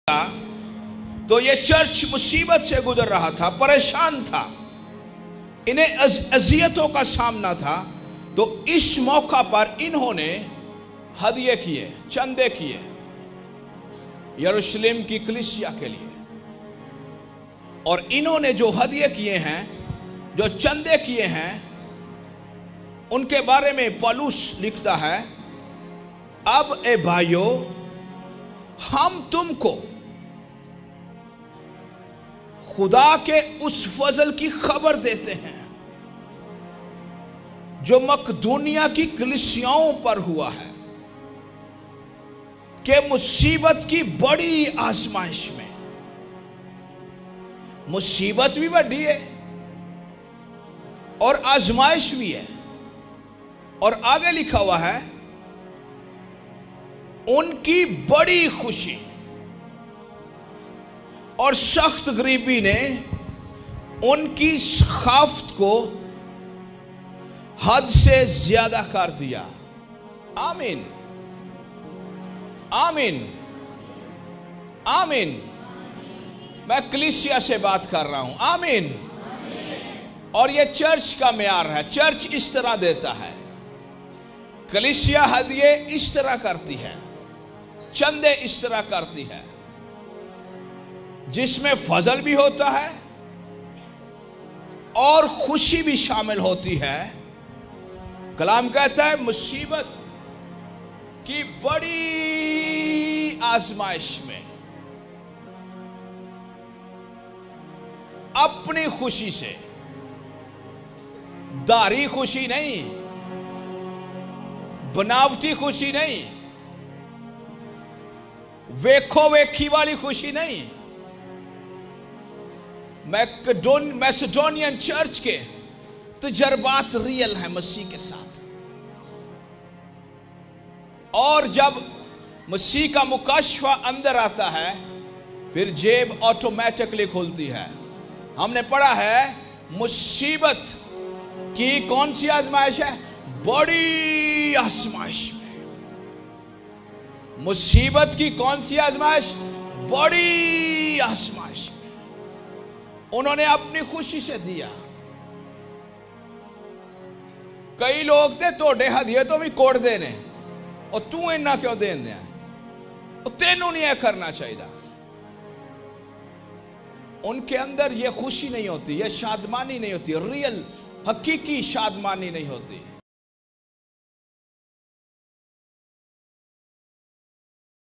Extended Hands In Suffering. 2-Cor 8:1-2
Macedonian Church was graced to give even in the fierce trail of persecution. And their giving attitude is a contour and specimen for the modern church to follow the footsteps. Blessings.